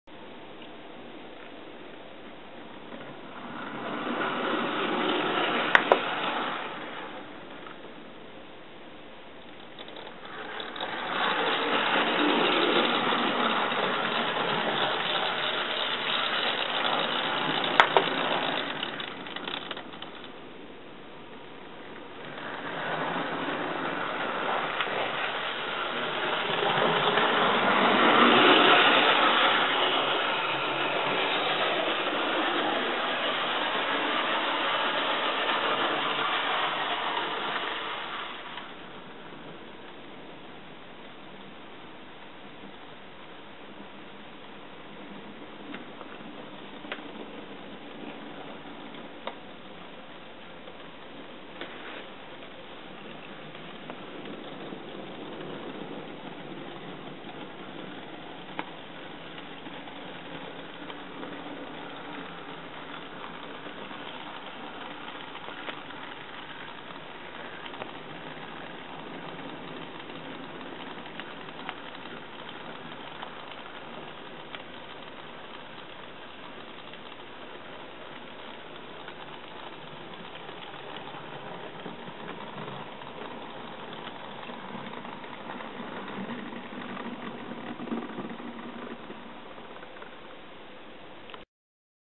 Bachman N Class DCC Conversion
This run shows the good slow run qualities of this model. A straight forward conversion with apparantly limited space but the dc circuit board can be removed to create room for the decoder on hard wiring